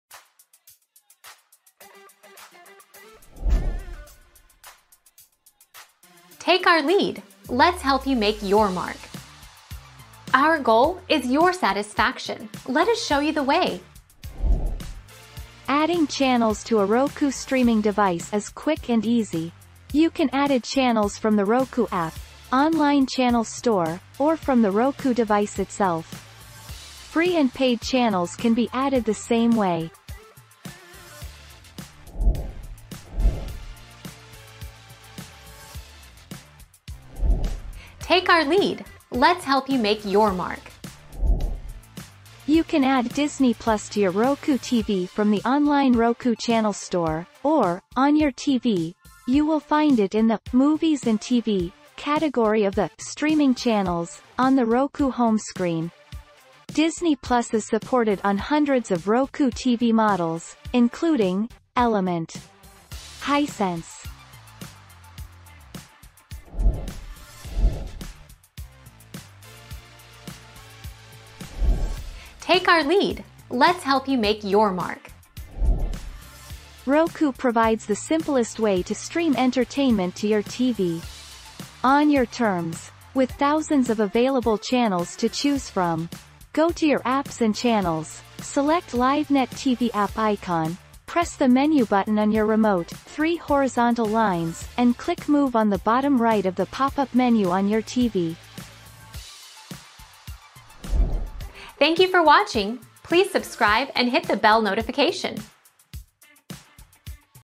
Can you download apps to Roku stick?
00:00 - Can you download apps to Roku stick?
00:34 - Does Roku support Disney plus?
01:08 - How do I download live TV on Roku?

Laura S. Harris (2021, January 7.) Can you download apps to Roku stick?
    AskAbout.video/articles/Can-you-download-apps-to-Roku-stick-203002